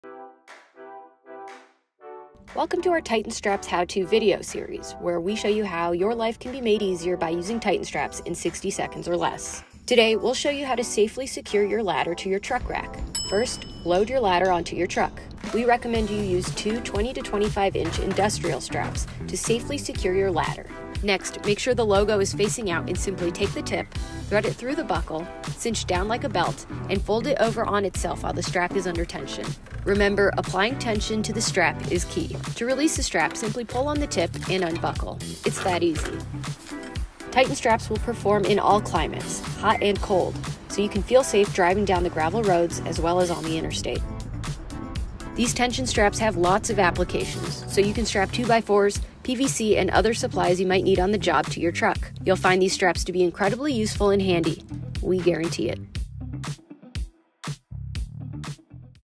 Titan Straps How-To Series: Lash Your Ladder to Your Truck Rack Safely & Securely w/ Tension Straps
Welcome to our Titan Straps How-To Series, where we show you how to use our tension straps on the job site, at home or in the field. In this video we show you how to use two 30" Industrial Straps to safely, securely and easily lash a ladder to your truck rack.